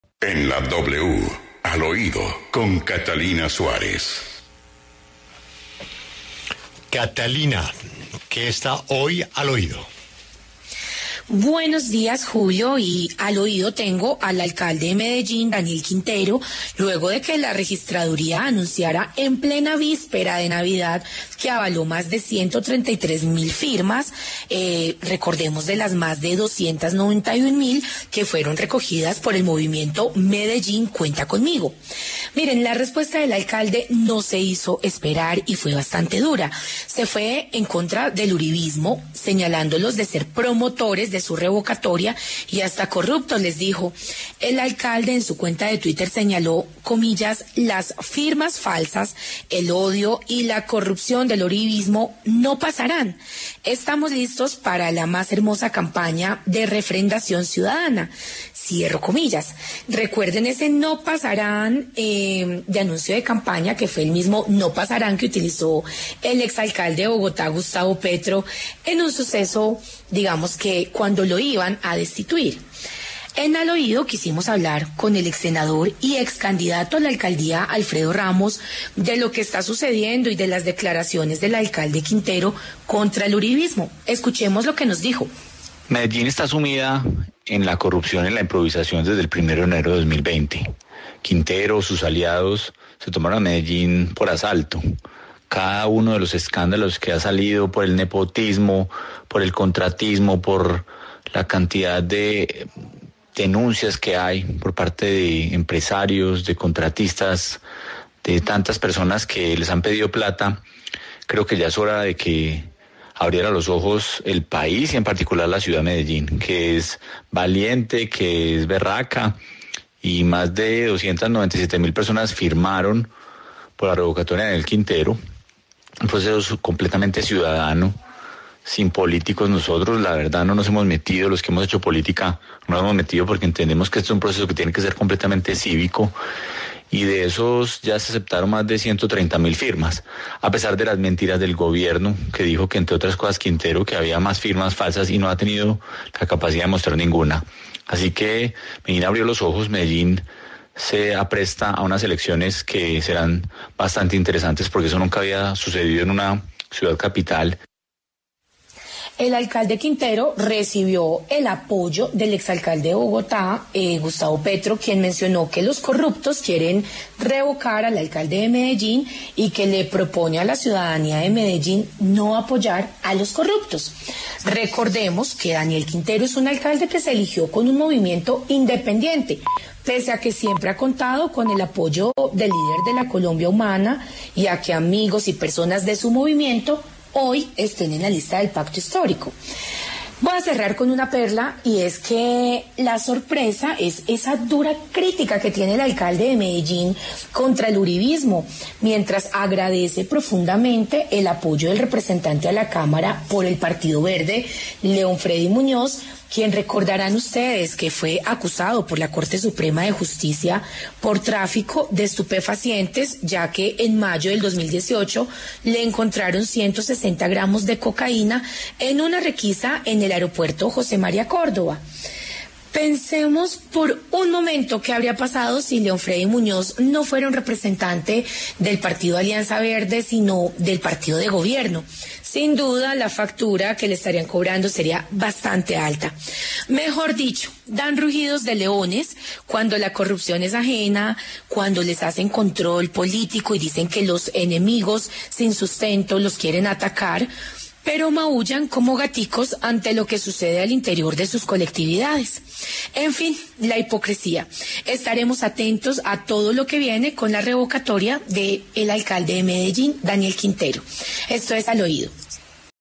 Revocatoria del alcalde Daniel Quintero, ¿el regalo de Navidad para Medellín?
Luego de que la Registraduría Civil de la Nación anunciara en plena víspera de Navidad que avaló más de 133.000 firmas, de las 291.000 que fueron recogidas por el movimiento “Medellín cuenta conmigo”, la respuesta del alcalde de Medellín, Daniel Quintero, fue dura y no se hizo esperar. Se fue en contra del uribismo.

“Las firmas falsas, el odio y la corrupción del uribismo no pasarán. Estamos listos para la más hermosa campaña de refrendación ciudadana”, manifestó Quintero en su cuenta de Twitter.